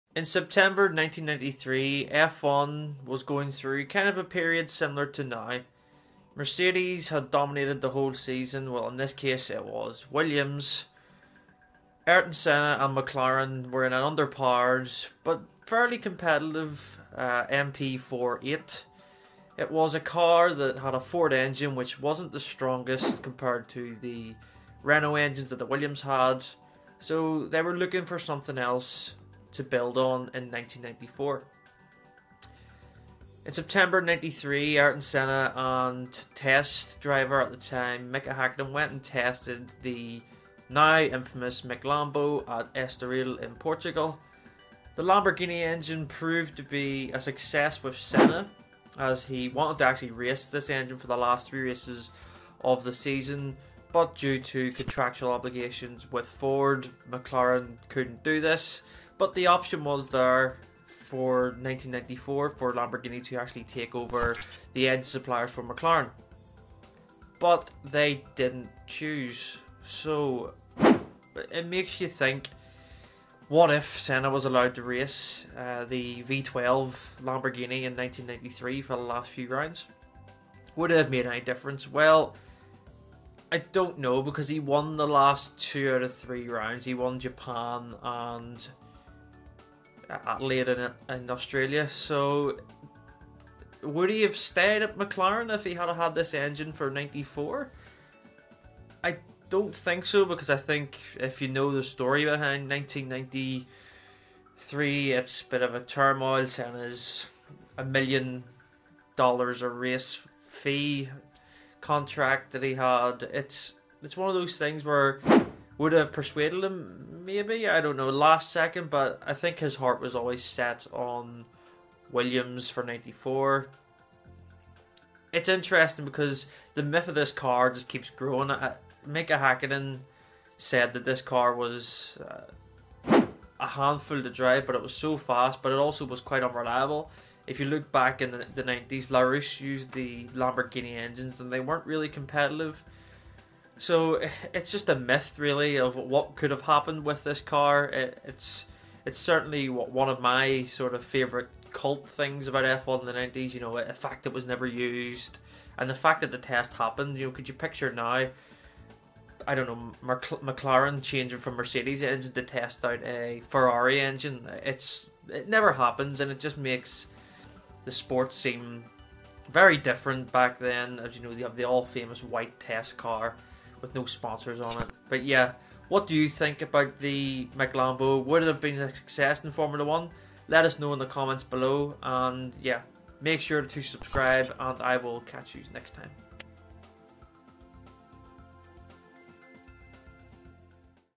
The McLambo - The engine that Ayrton Senna loved - F1 1993
In 1993, McLaren tested the V12 Lamborghini engine and Ayrton Senna immediately liked it. What would have McLaren been like in 1994 if they choose the Italian outfit, could Senna have even stayed for 1994? We'll never know.